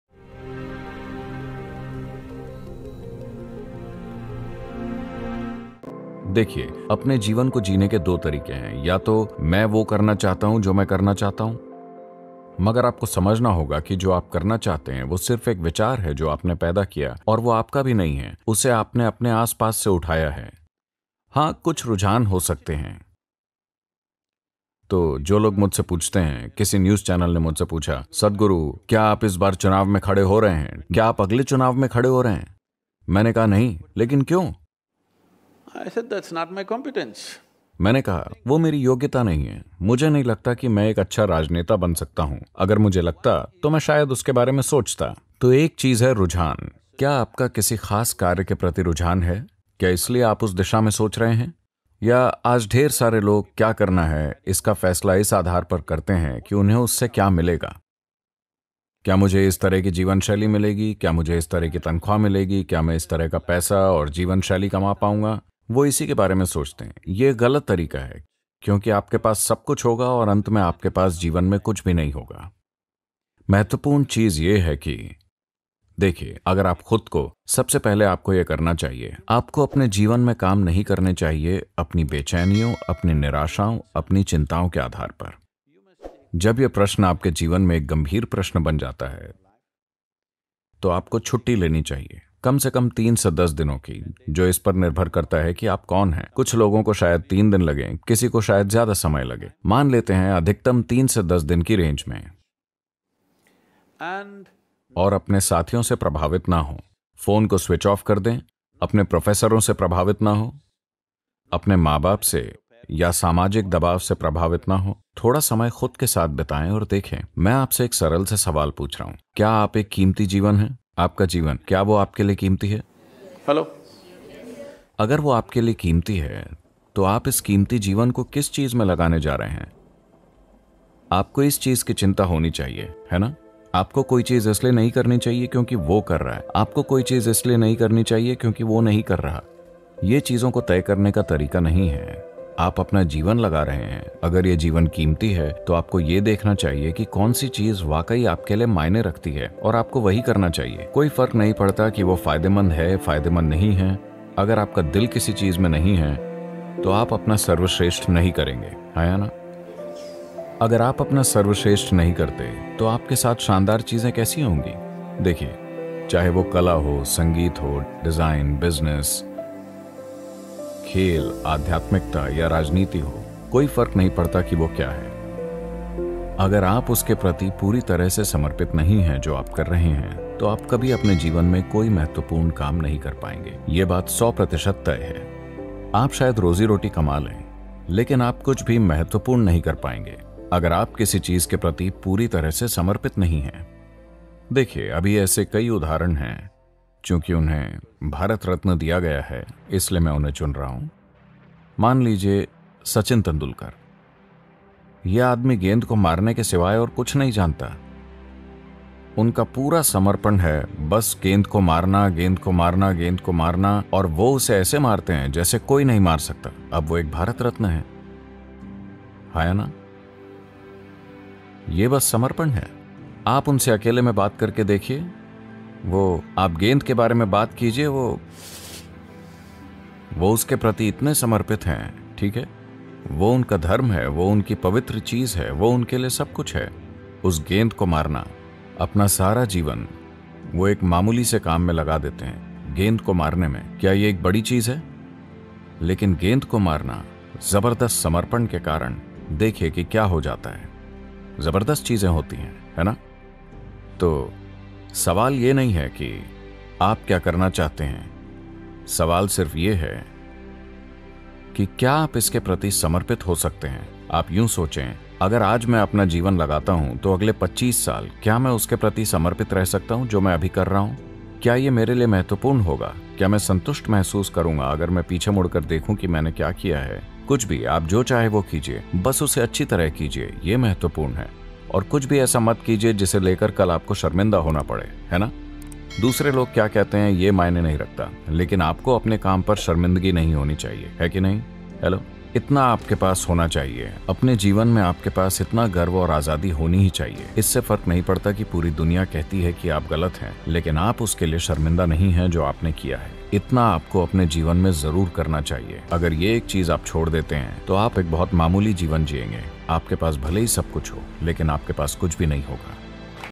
यह एक गुण आपको सफल बनाएगा | One quality will make you successful | Success Tips | Sadhguru Hindi
बस एक चीज करलो सफलता जरूर मिलेगी | Just do one thing you will get success | Sadhguru Sandesh Hindi

1) This video has no negative impact on the original works
2) This video is also for teaching purposes.
3) Original Content is transformed to create unique Videos
4) I only used bits and pieces of videos to get the point across where necessary.